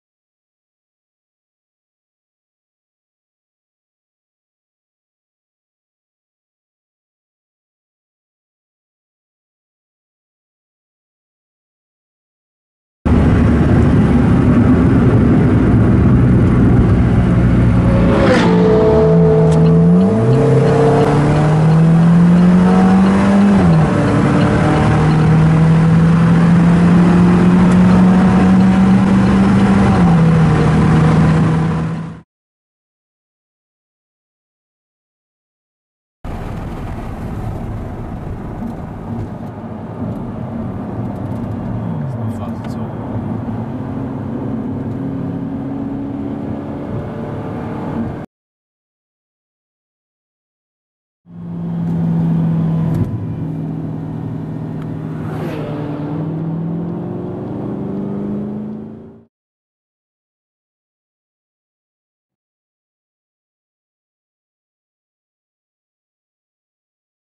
High Speed Armytrix Audi R8 vs Alfa Romeo 4C Fly-Bys!
I LOVE THE AUTOBAHN! Myself in the R8 vs Sam (SeenThroughGlass)! Epic Epic Short Video!